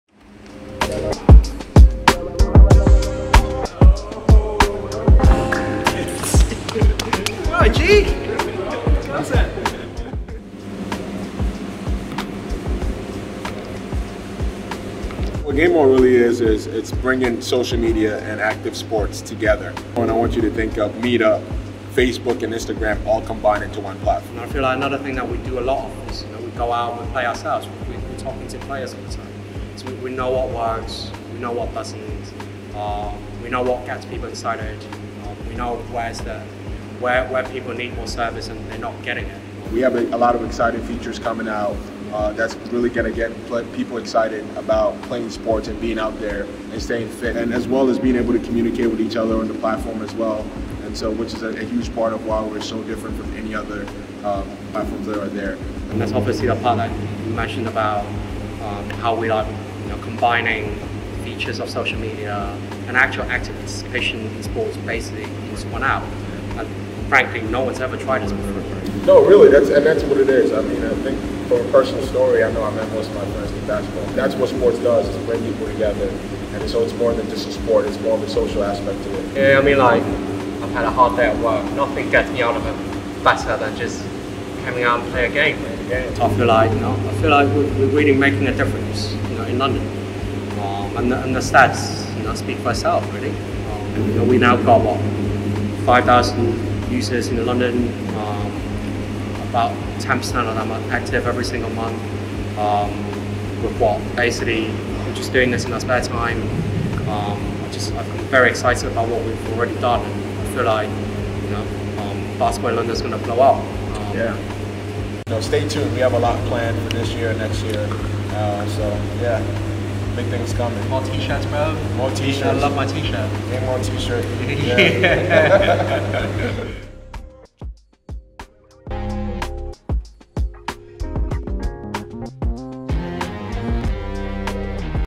Meet the GameOn Active Founders
We created GameOn Active because we want to see more people stay active in their busy lives by taking part in the sports or physical activities that they love. For now, we are focused on the basketball community in the UK but we will look to expand to other sports and locations over time. To help us grow we are always looking for more organizers, courts, referees and players - do get in touch if you would like to help or work with us!